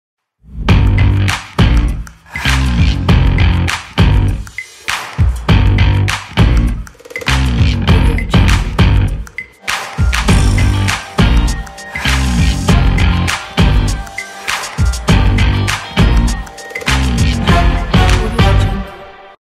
Urban Style Opener
This Opener is a great slideshow styled for dynamic photo or video presentation for urban style promo

FEATURES

    No plugins
    Easy customization Text and Logo
    Resolution: Full HD 1920×1080p
    Modular structure
    PDF Tutorial included
    Free font used – link included
    9 Media holders
    1 Logo holder
    16 Text Holders